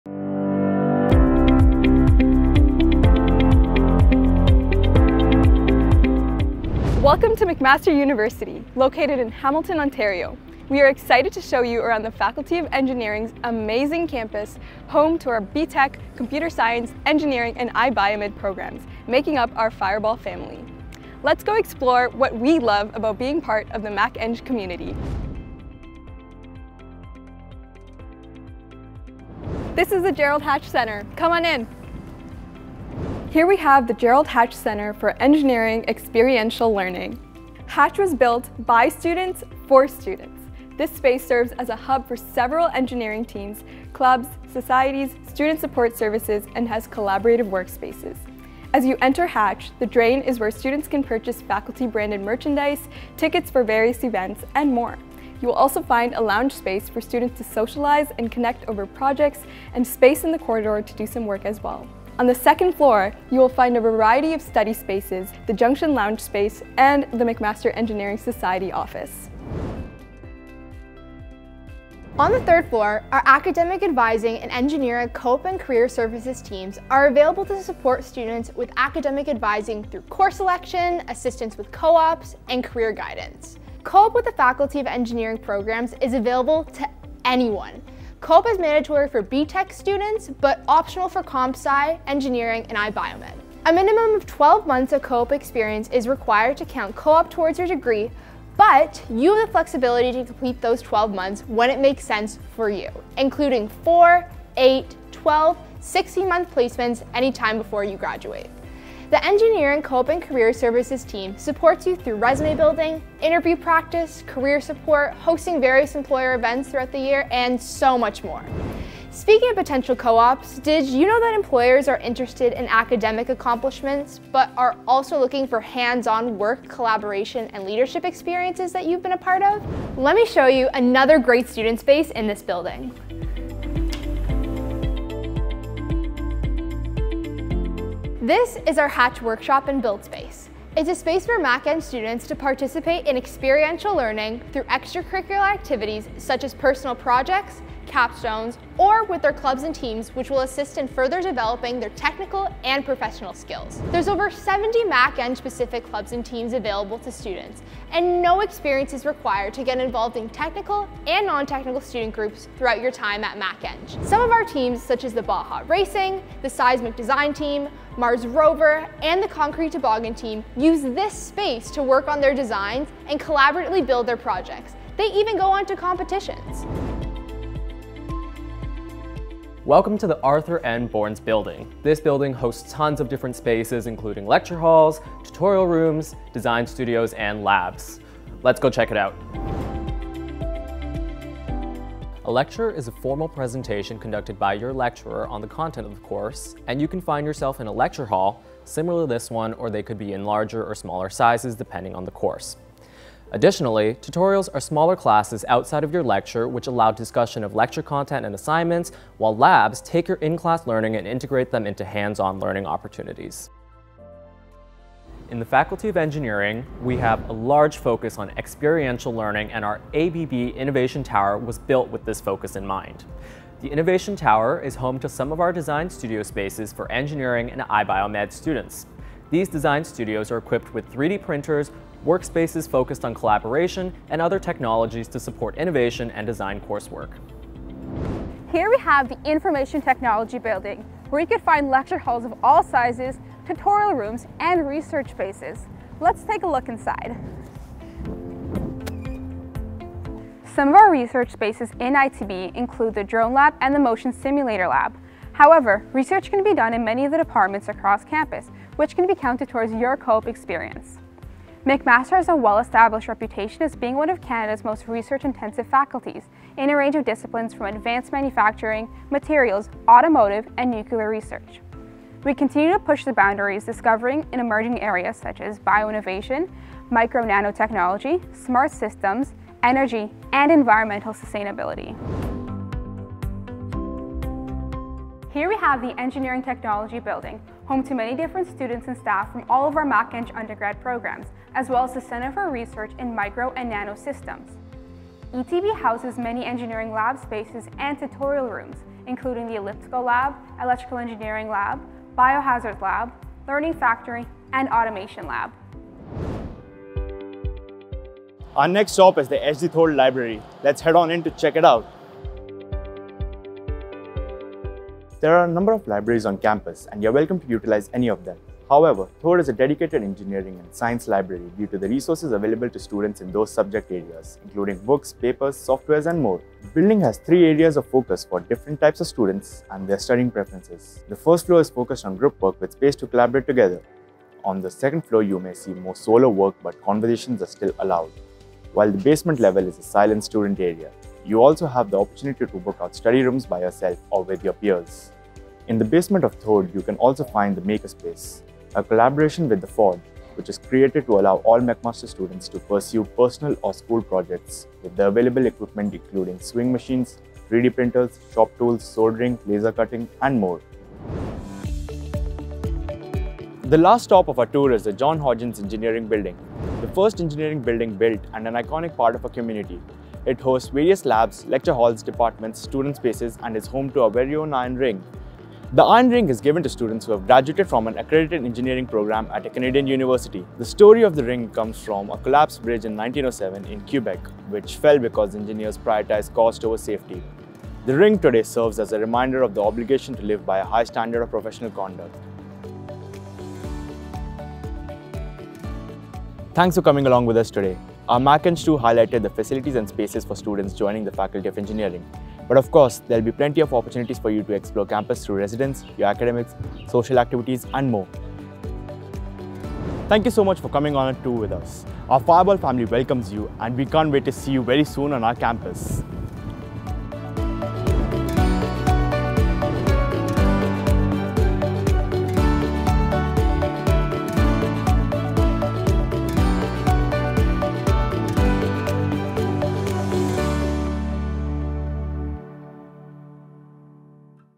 Take a Mac Eng Tour | McMaster Engineering
Take a tour of the Faculty of Engineering at McMaster University with a couple of our MacEng students to learn more about our buildings, facilities, and student spaces. We are excited to show you around.

Questions?
You can also connect with our current students on our online chat platform here.